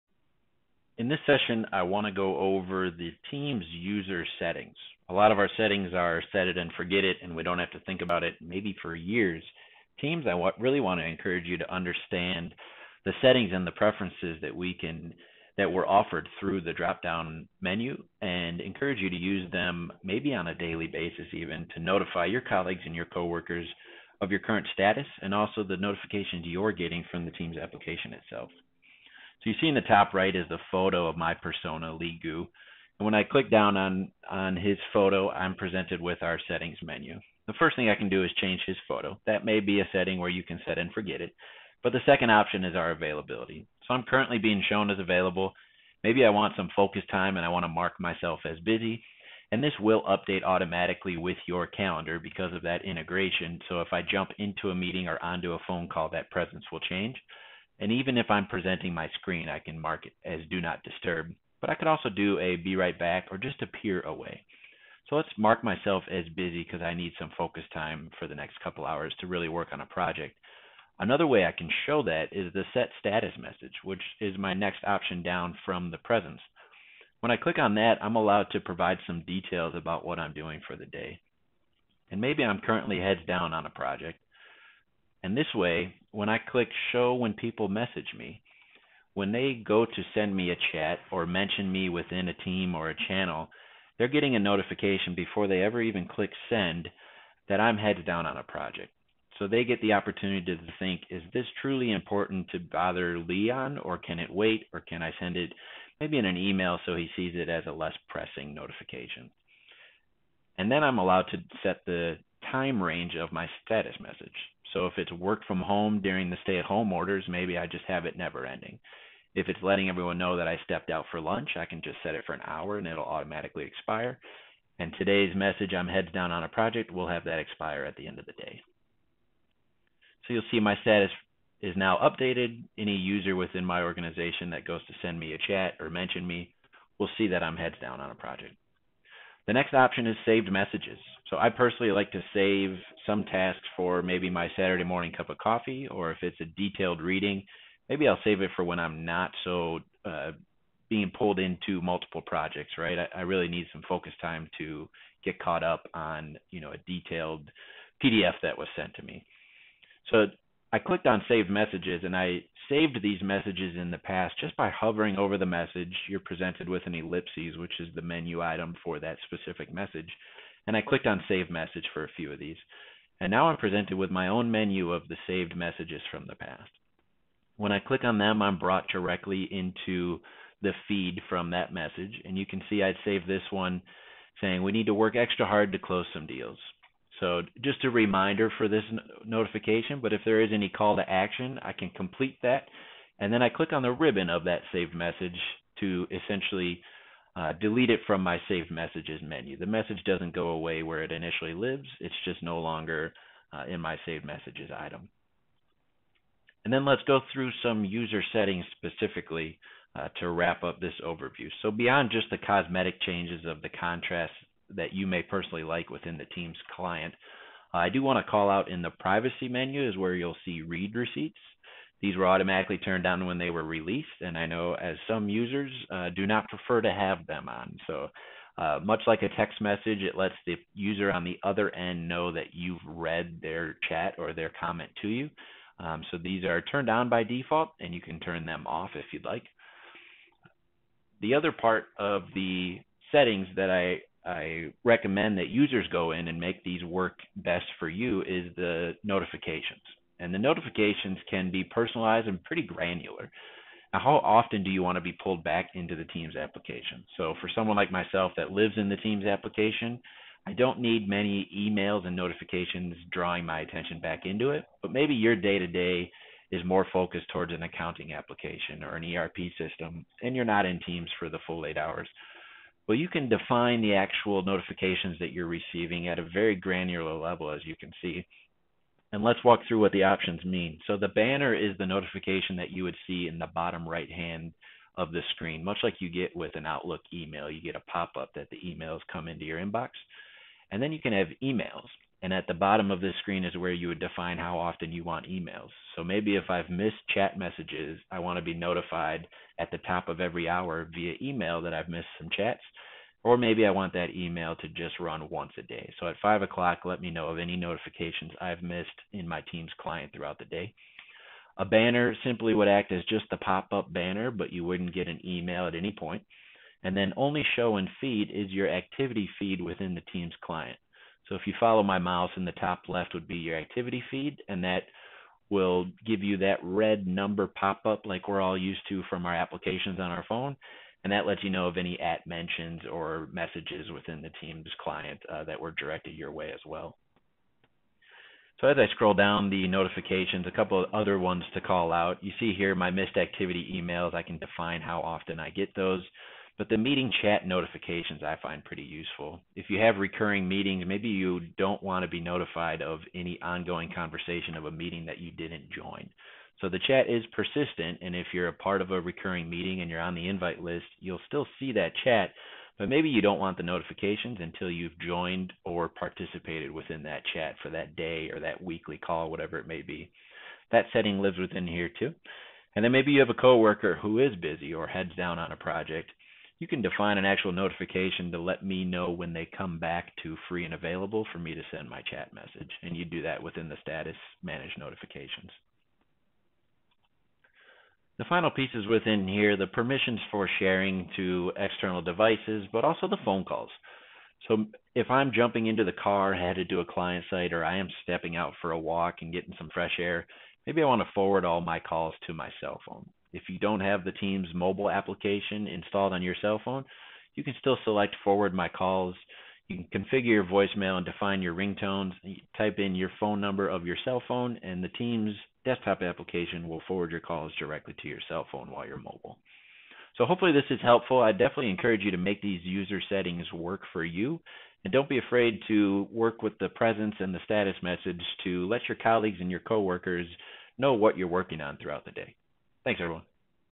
User Settings (Presence, Status, Saved Messages, Notifications)
Settings in Teams aren't just ‘set it and forget it.' We discuss why you may see the need to be in the settings menu on a daily basis – and how to make the most of your Teams settings and notifications.

Interested in learning more?